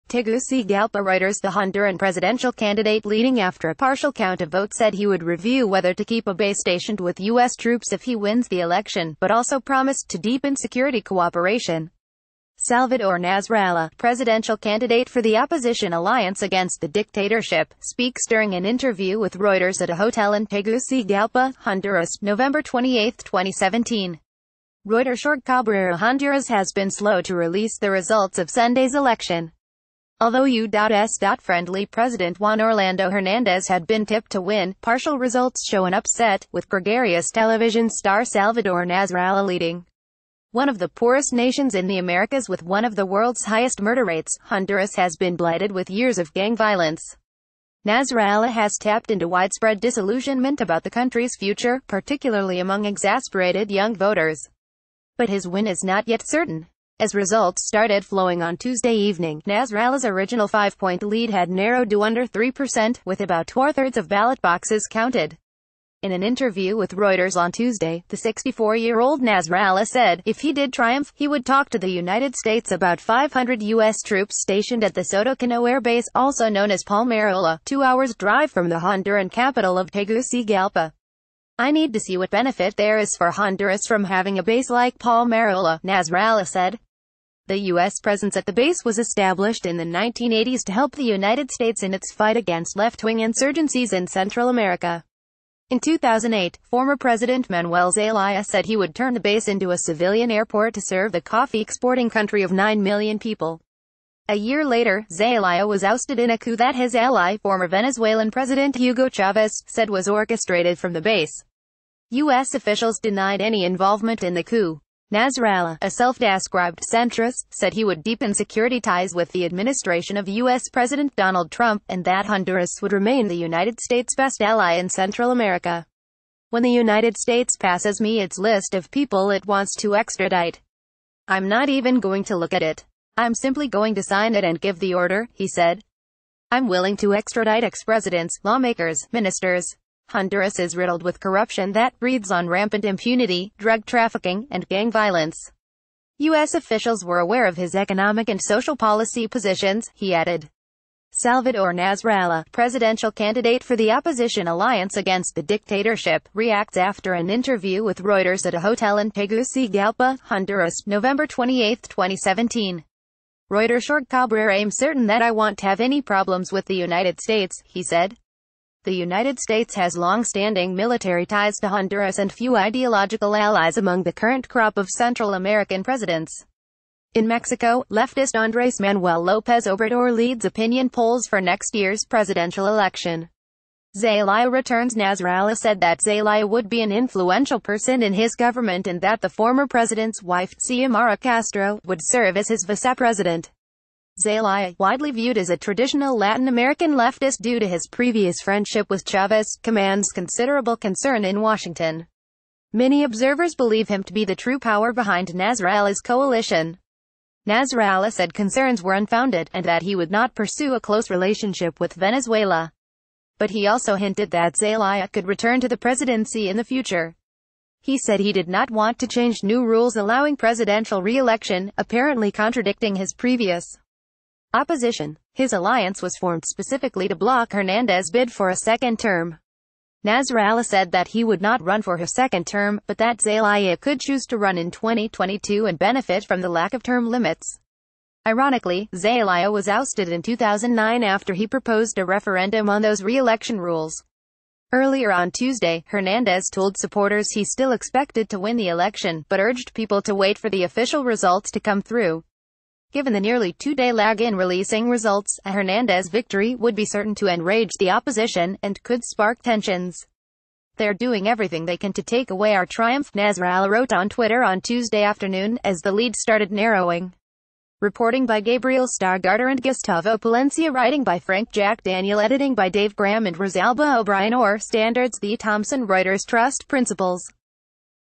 News-the Honduran presidential candidates to review the U.S. military presence
News - News-the Honduran presidential candidates to review the U.S. military presence

TEGUCIGALPA Reuters The Honduran presidential candidate leading after a partial count of votes said he would review whether to keep a base stationed with U.S. troops if he wins the election, but also promised to deepen security cooperation. Salvador Nasralla, presidential candidate for the Opposition Alliance Against the Dictatorship, speaks during an interview with Reuters at a hotel in Tegucigalpa, Honduras, November 28, 2017. REUTERSJorge CabreraHonduras has been slow to release the results o...